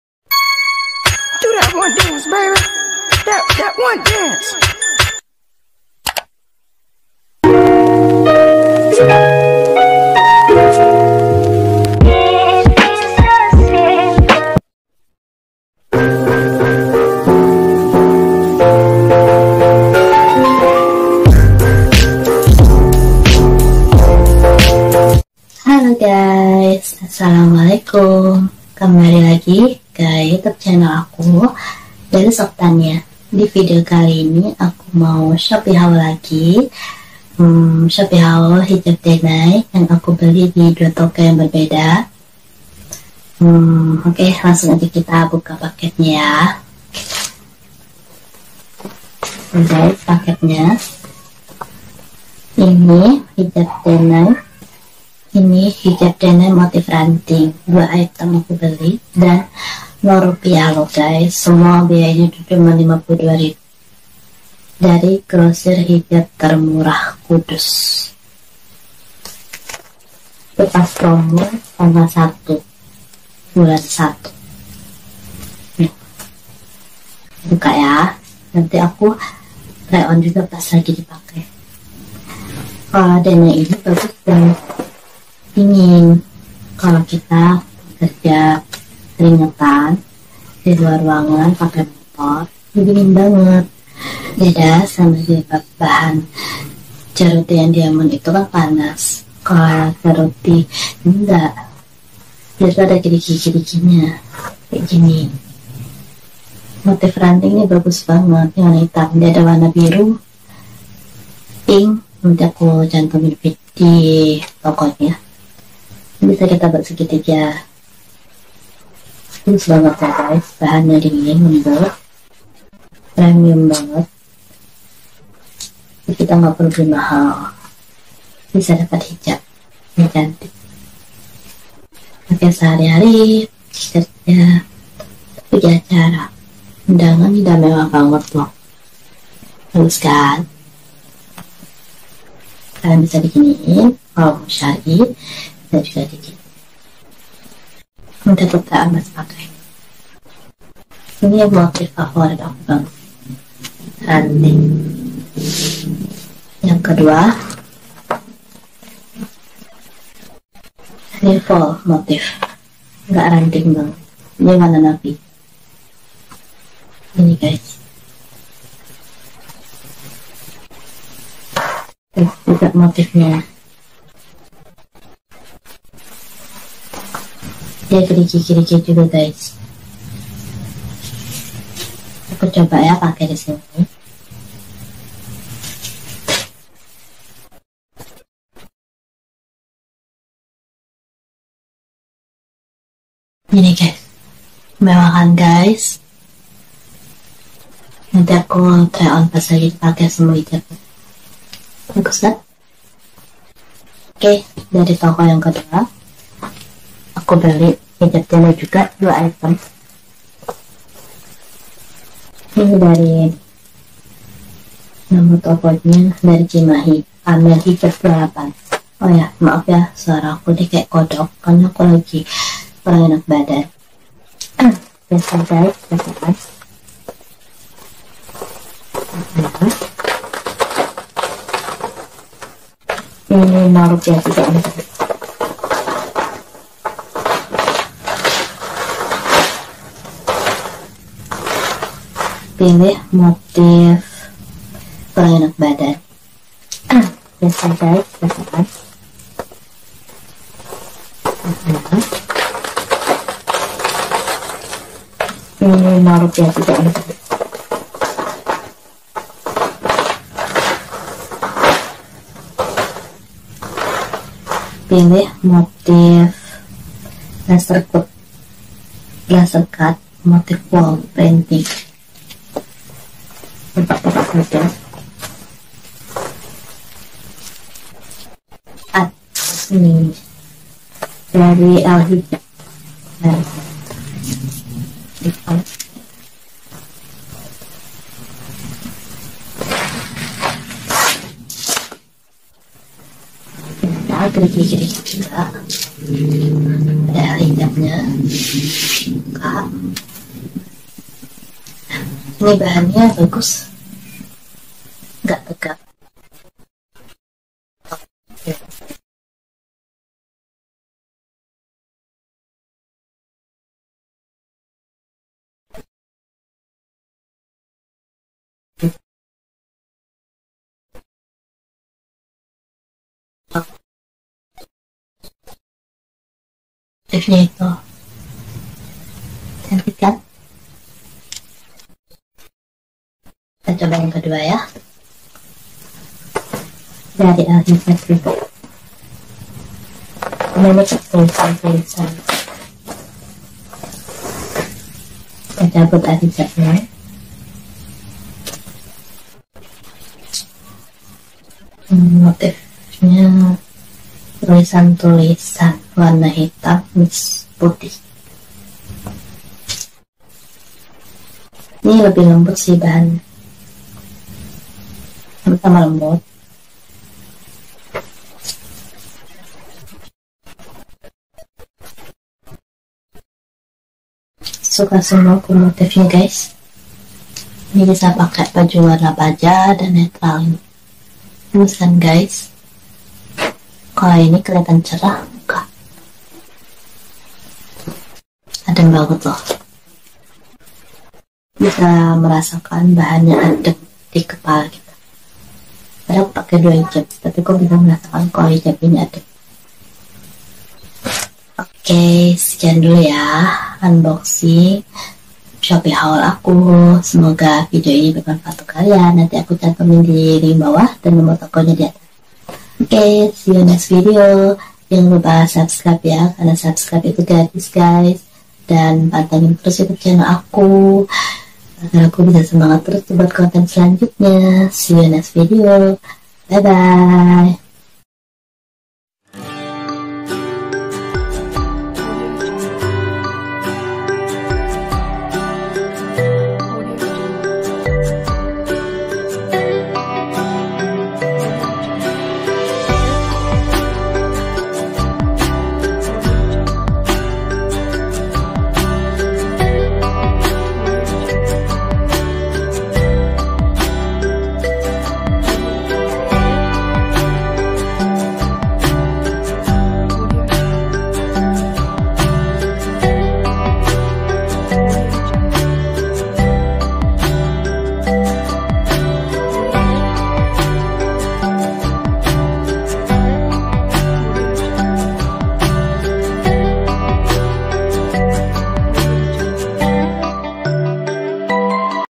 SHOPEE HAUL HIJAB DENAY MOTIF ABSTRAK STYLE HIJAB UNTUK OUTFIT LEBARAN IED FITRI
Hay guys aq mau rekomendasikan hijab denay yg kekinian dengan motif abstrak beli di shoppe produk nya
Ini link nya :